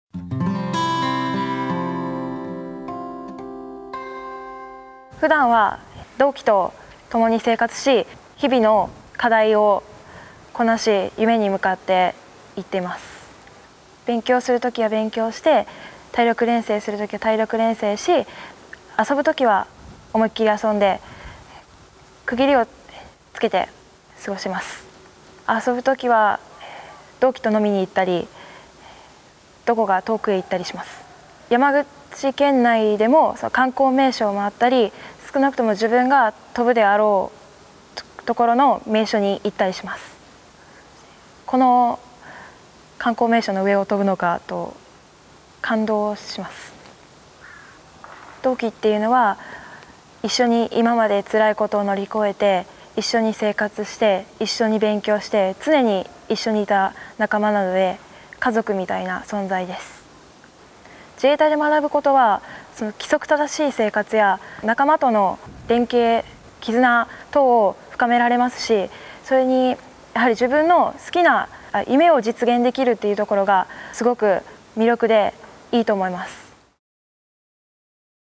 先輩Q&A：どんな生活？ 航空学生
航空学生の出身者にインタビュー。普段の生活の様子を聞いてみました。